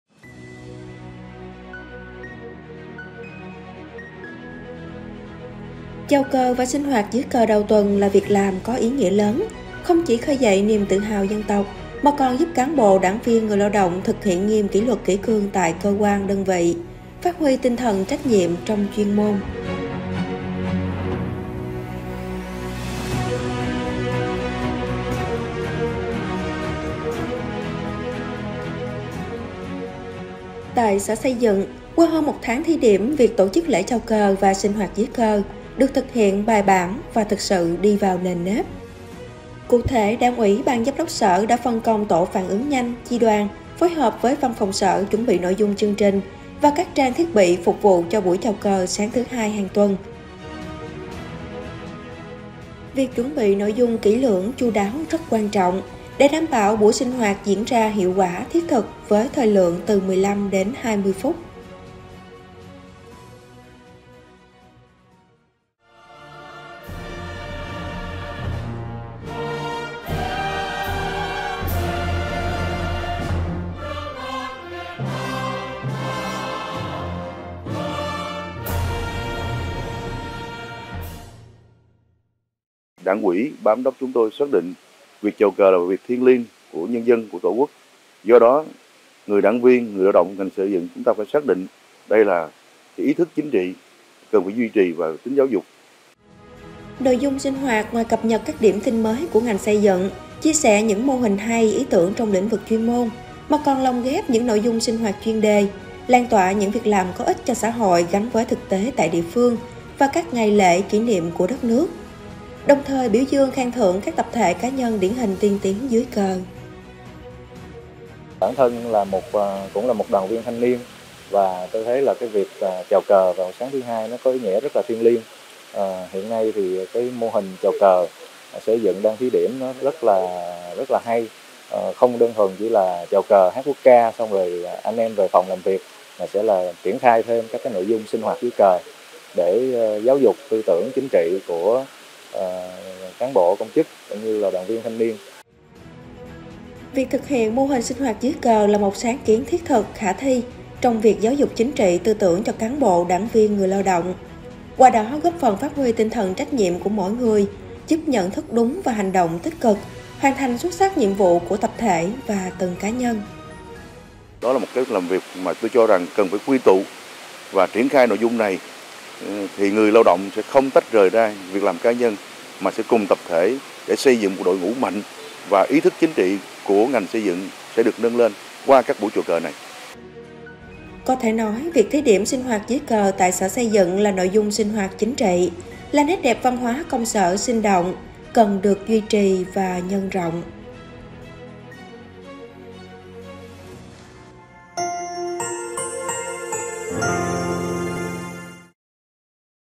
Sinh hoạt dưới cờ tại Sở Xây dựng: Nét đẹp văn hóa, chính trị cần nhân rộng
Từ tháng 4 vừa qua, Sở Xây dựng triển khai thí điểm sinh hoạt dưới cờ vào sáng thứ Hai hàng tuần sau buổi chào cờ. Sau gần 1 tháng thí điểm, các buổi sinh hoạt dưới cờ tại Sở Xây dựng đã diễn ra trang trọng, đi vào nền nếp, mang lại hiệu quả thiết thực.